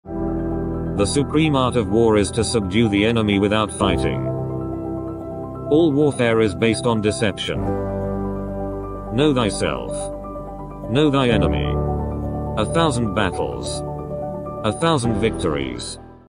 3 Best Quotes From Sun Tzu | AI Animated Photo 🤯💪
28 Apr 2023 3 Best Quotes From Sun Tzu with AI Animating Photo Talking.
1. "The supreme art of war is to subdue the enemy without fighting."
2. "All warfare is based on deception."
3."Know thy self, know thy enemy. A thousand battles, a thousand victories."